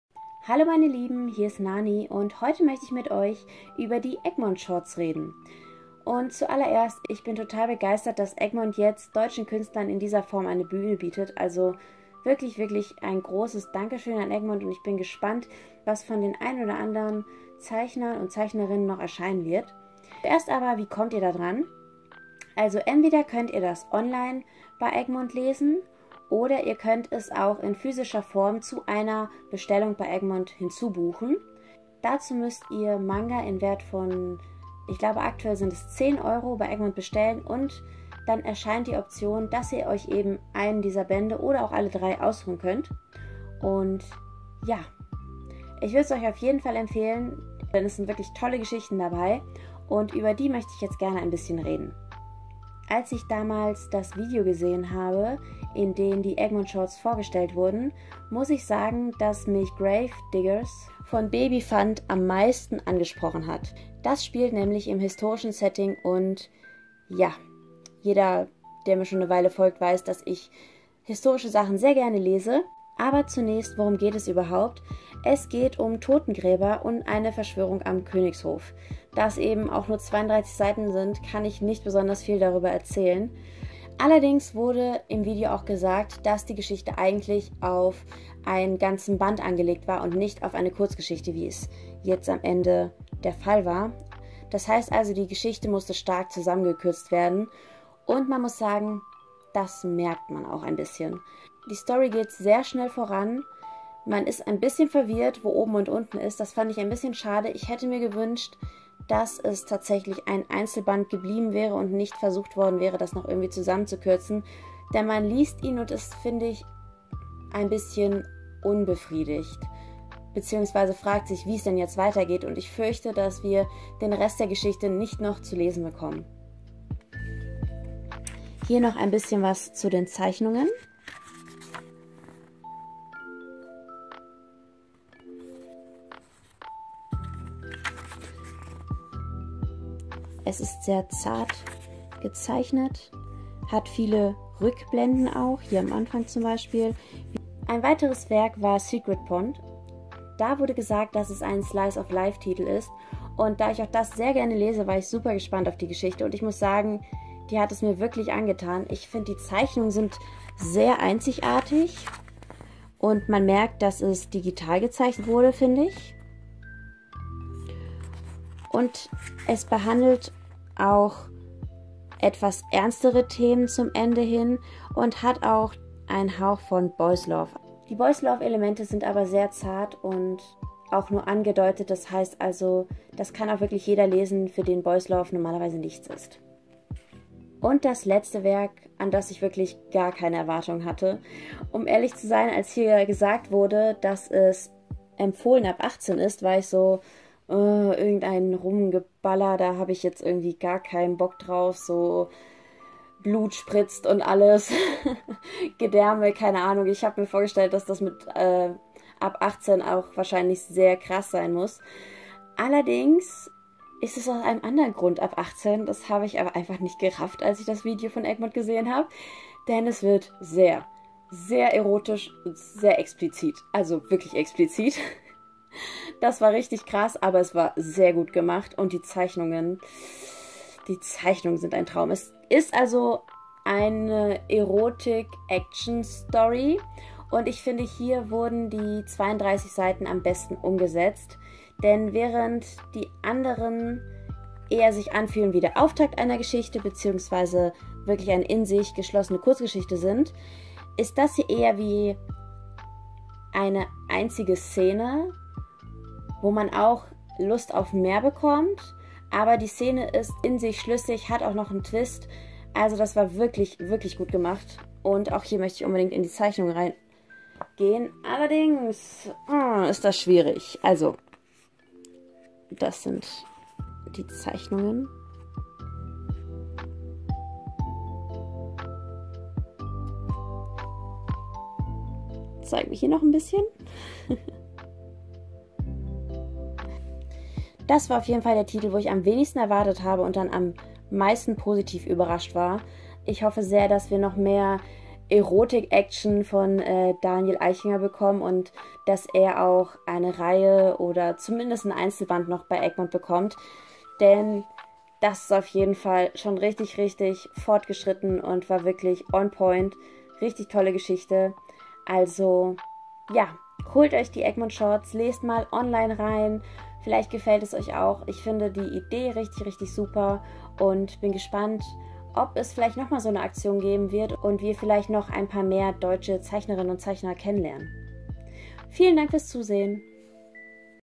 Egmont Shorts (GRAVEDIGGERS, SECRET POND & OVENTROJA LOVESTORY) || Manga Rezension
Diesmal eine ganz besondere Review! Ich stelle euch kurz die Egmont Shorts vor und gebe euch meine Einschätzung dazu :3

0:00 Kurze Vorstellung der Egmont Shorts
0:52 GRAVEDIGGERS von babyfant
2:26 SECRET POND von REN M. PAPE
3:07 OVENTROJA LOVESTORY von Daniel Eichinger

Eure Nani